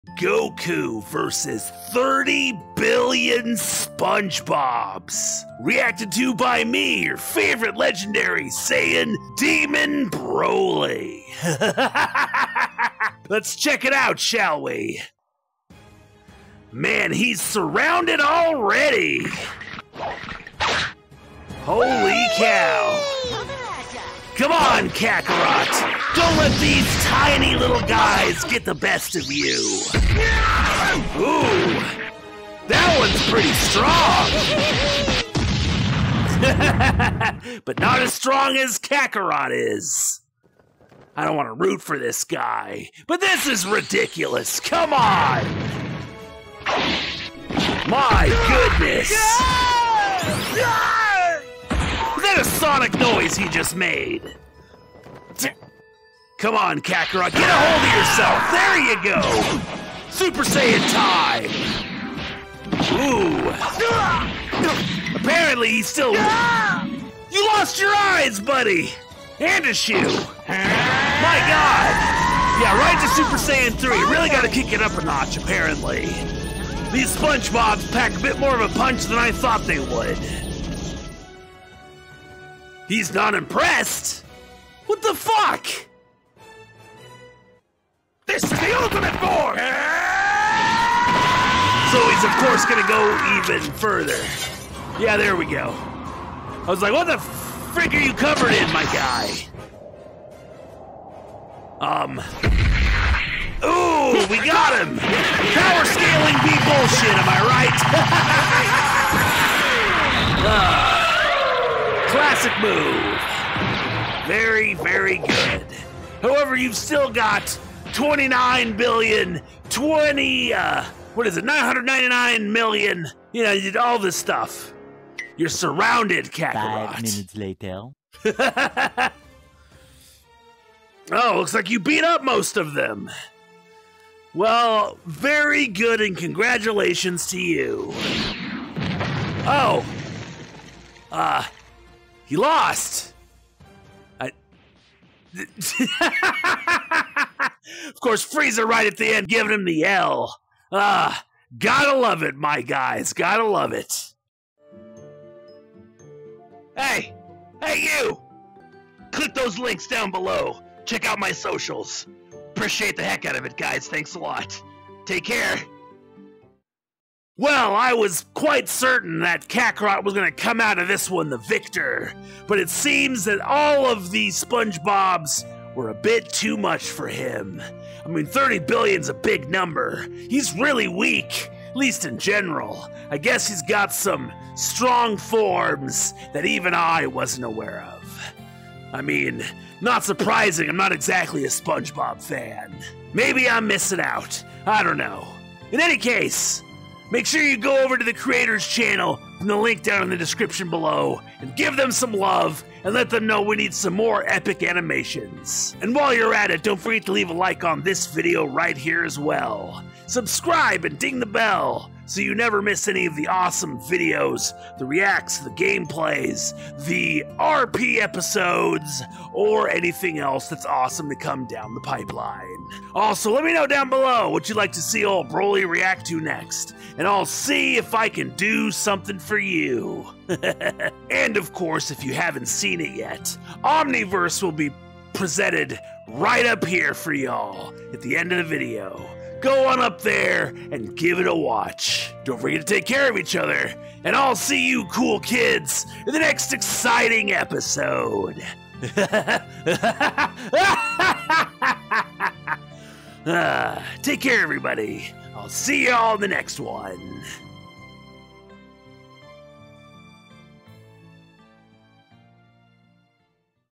GOKU VS 30 BILLION SPONGEBOBS (ANIMATION) | Broly Reacts!
In one corner, Goku from Dragon Ball Z. In the other, 30 Billion Spongebobs! Has goku met his greatest challenge yet? Will he win this time? Broly reacts to the intense battle!